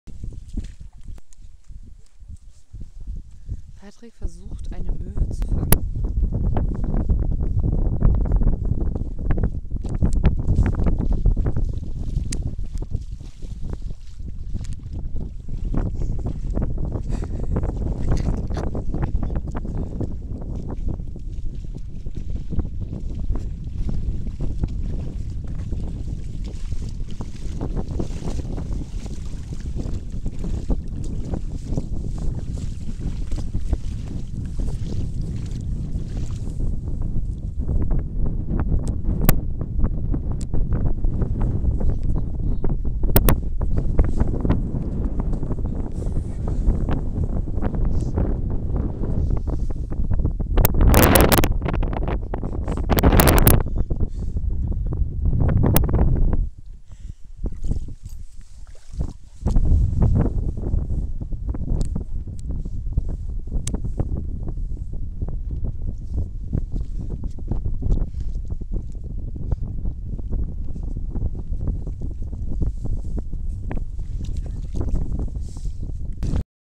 Möwenjagd
Wattwanderung beim Bundesjugendtreffen Südwest trifft Küste. 2013 Cuxhaven